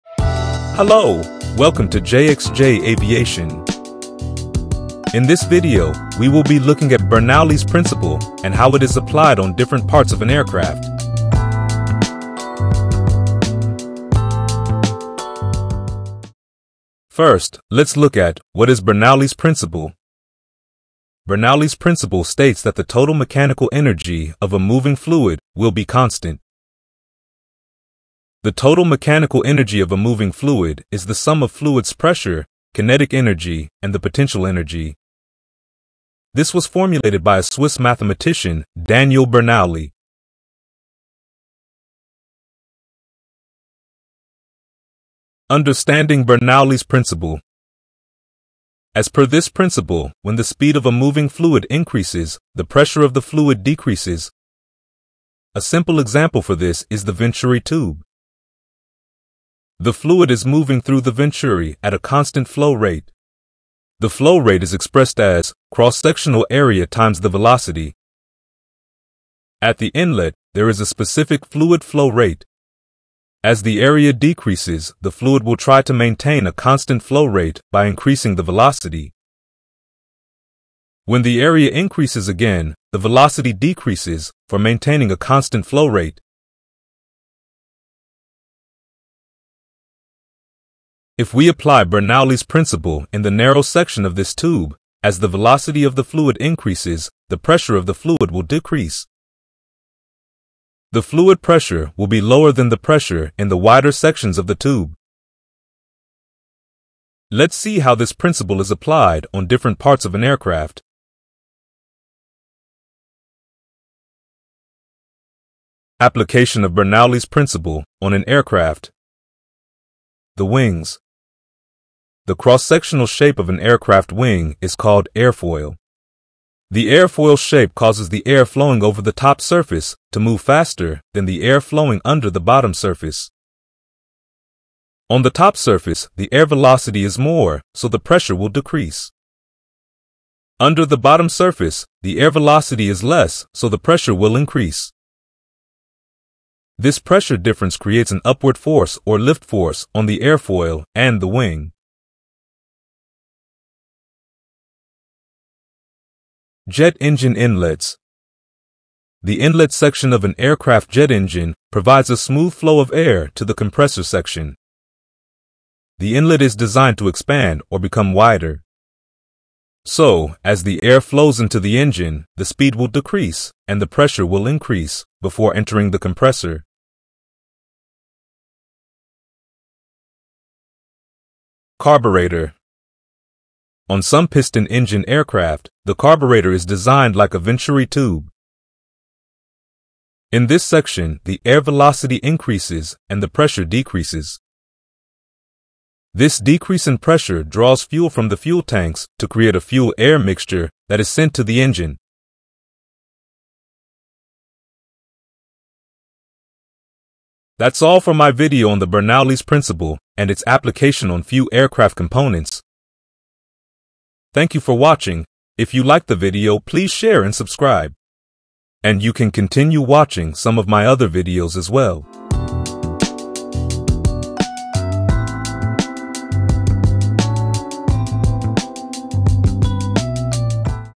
Understanding Bernoulli's Principle | Application on Aircraft: Wings, Engine Inlet and Carburetor
Hi. In this video we look at what is Bernoulli's Principle and the relation it gives between Velocity and Pressure. We see the best example for it: the venturi tube. The tube has a change in area, where the velocity will change to maintain a constant flow rate and accordingly the pressure changes as well.

We also look at a few application of this principle on an aircraft, mainly the wing, engine inlet and the carburetor on few piston engine aircraft.